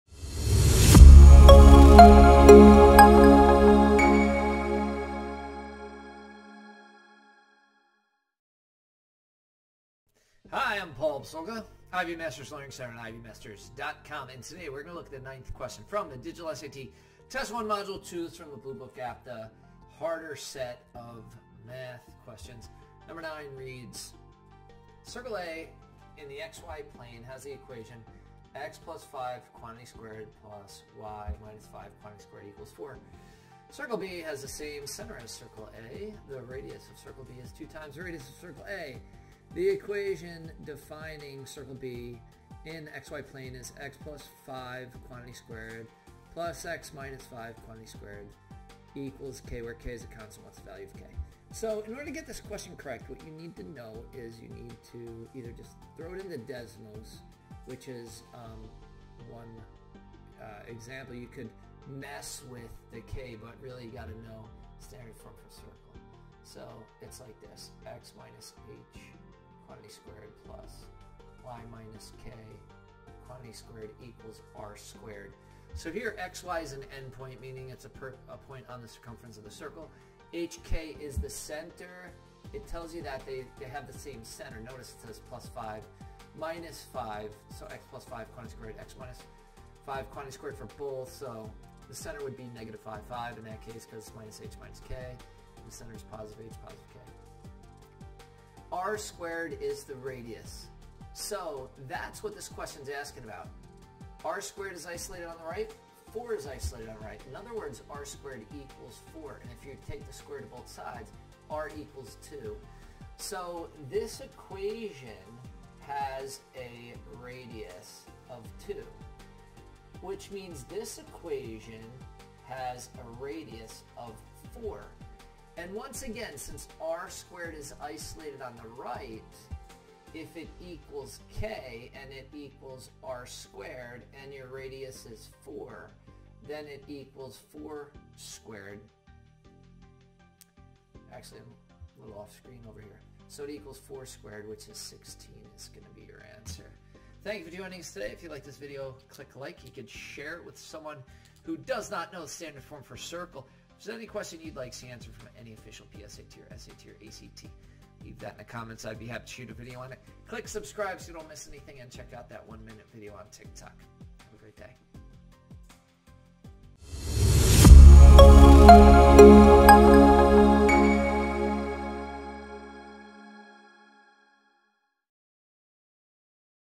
Digital SAT practice test 1 module 2 number 9 harder math || Ivy Masters
Struggling with harder SAT Math questions? Watch this video to tackle Module 2, Question #9 from a practice test designed by Ivy Masters. Enhance your digital SAT Math skills and improve your test-taking strategies with this challenging problem-solving session. Stay ahead of the competition with Ivy Masters!
Standard form of a circle

(x - h)^2 + (y - k)^2 = r^2, r is the radius, You could pick a number here, Bluebook app, Blue Book app, new SAT

Circle A in the xy-plane has the equation (x + 5)^2 + (y - 5)^2 = 4. Circle B has the same
center as circle A. The radius of circle B is two times the radius of circle A. The equation
defining circle B, in the xy-plane is (x + 5)^2 + (y - 5)^2 = k, where k is a constant. What
is the value of k?